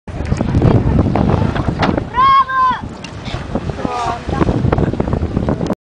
Bike Diving
A guy rides over a ramp to dive in the sea